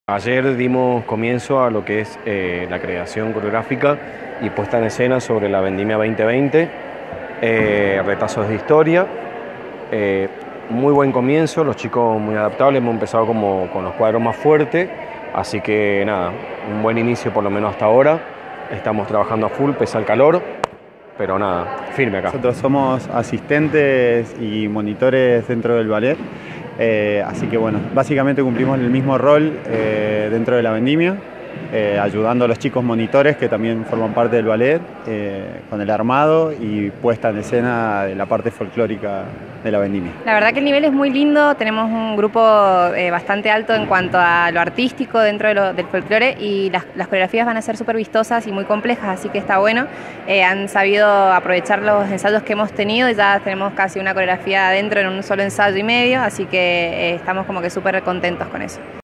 Ensayos de Folclore - Vendimia Las Heras 2020 “Retazos de historia”
Bajo la conducción de Hugo Luna, y el apoyo del Ballet Estable Municipal, comenzaron los ensayos de folclore en el Estadio Vicente Polimeni de cara a lo que será la máxima celebración del departamento el 17 de enero en el Parque de la Niñez.